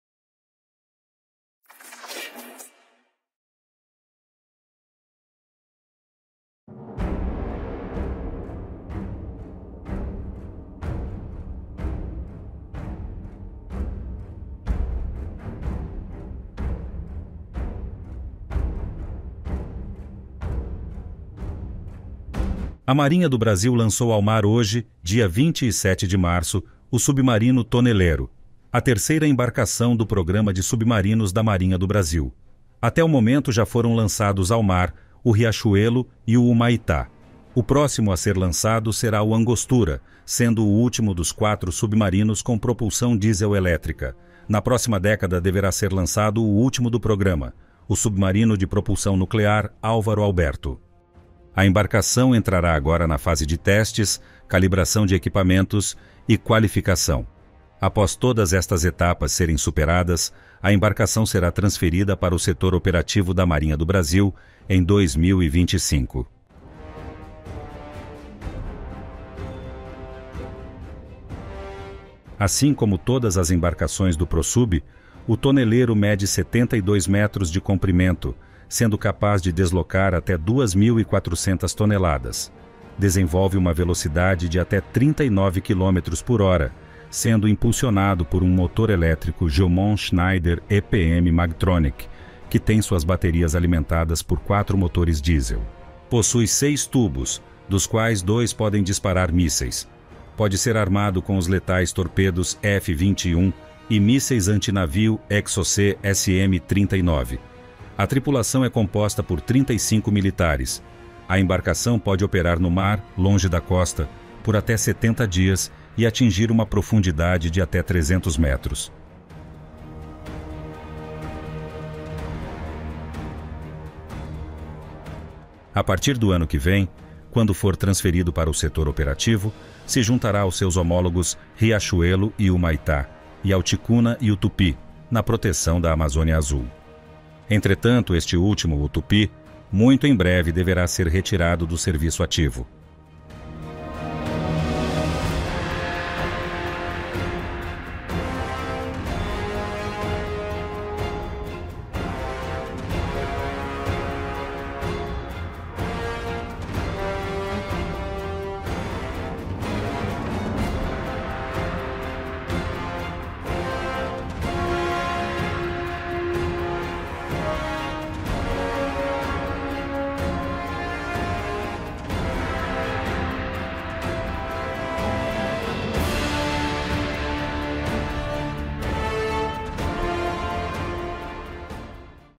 Lançado com sucesso! Submarino Brasileiro, o Tonelero (S42), é lançado ao mar.
A Marinha do Brasil lançou ao mar no dia de hoje 27 de março de 2024, o terceiro submarino do PROSUB, o Tonelero (S42). A cerimônia contou com participação de autoridades, como o presidente do Brasil e presidente da França. A embarcação entrará agora na fase de testes.